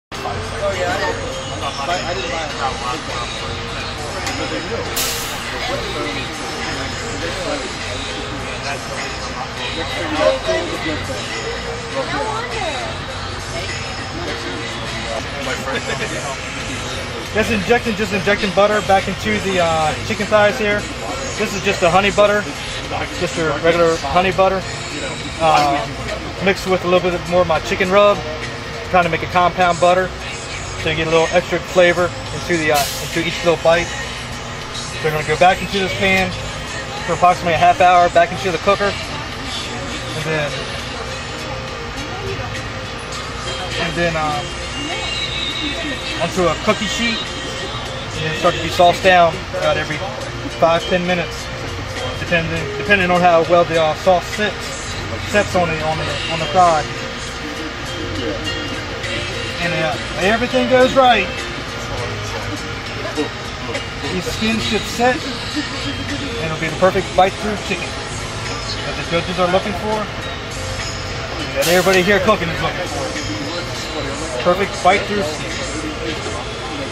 C M60 (Library) DJ Watkins Shows Us How To Make Chicken On A Lang 60 Deluxe
Chef DJ Watkins describes how he prepares his chicken for his Lang smoker, injecting the pieces with butter to ensure a flavor-filled bite throughout all of the pieces of chicken; a perfect complement to their quality smoking on the Lang cooker.